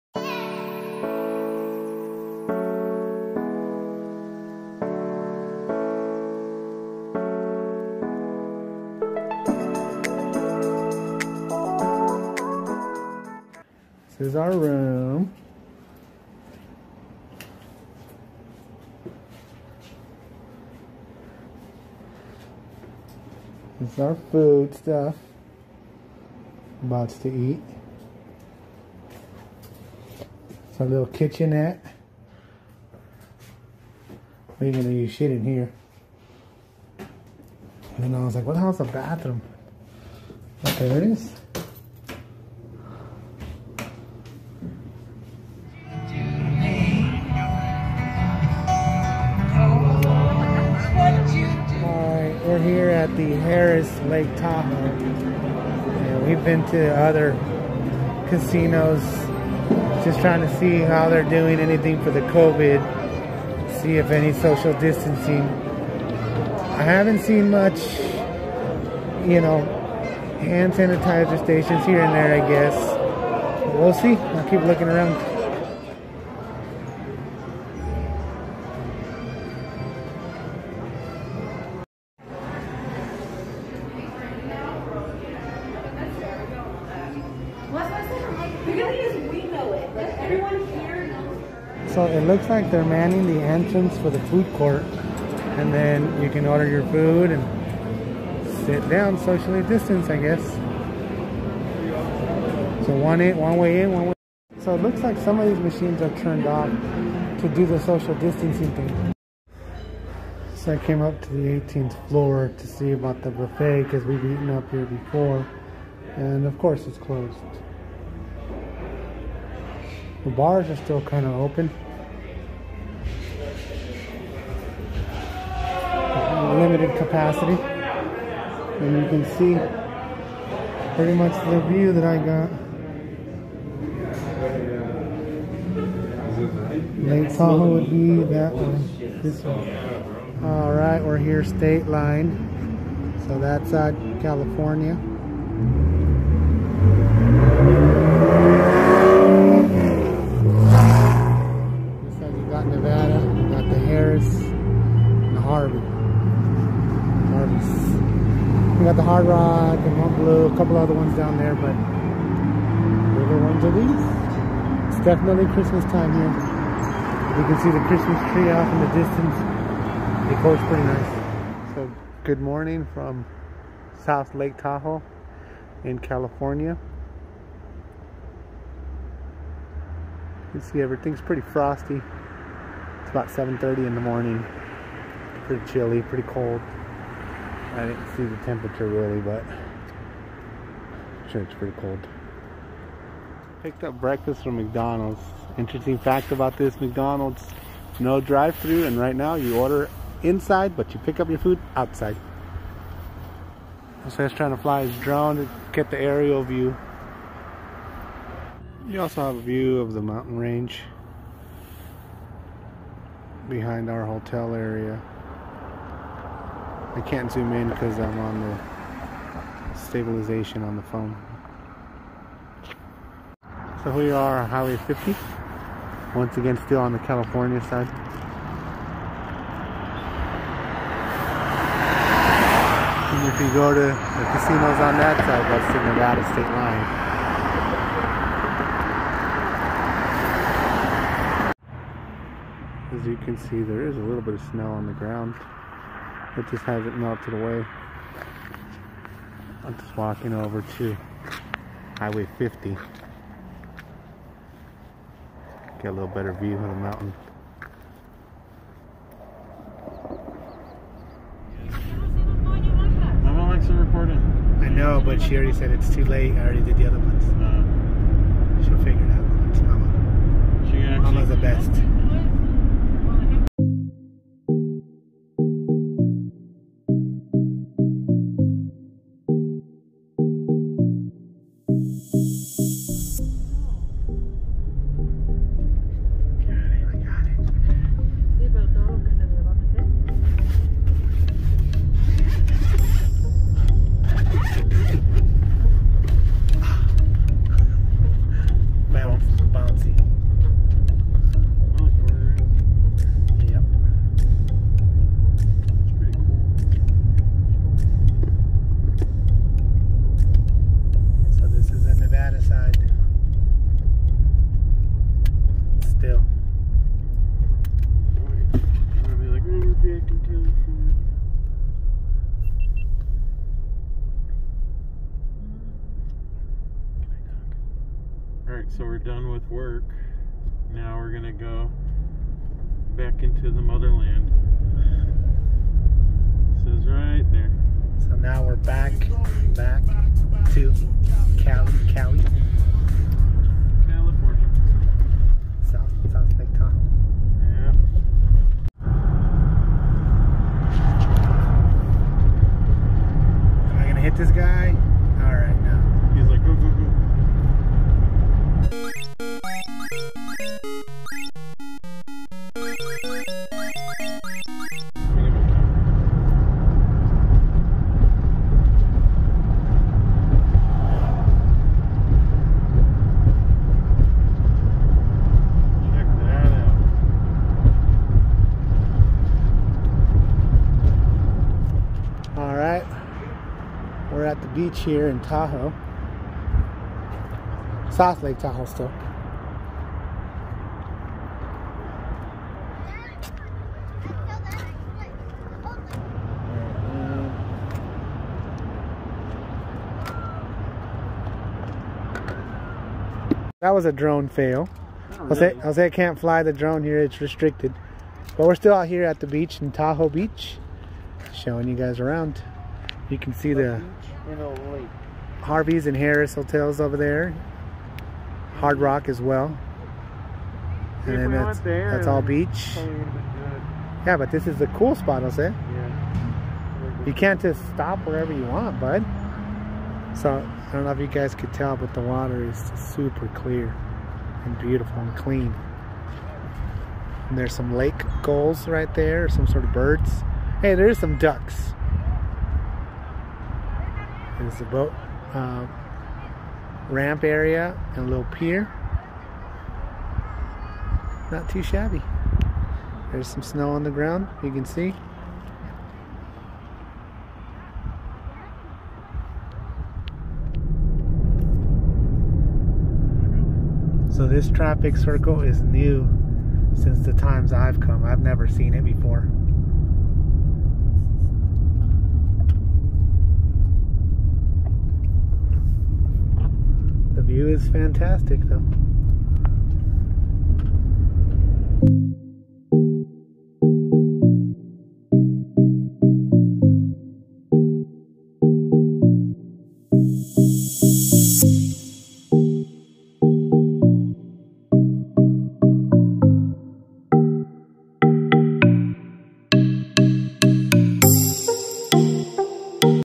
Kikos 1 night/day in South Lake Tahoe
Kiko had a quick work trip to South Lake Tahoe, of course he went to check out the buffet!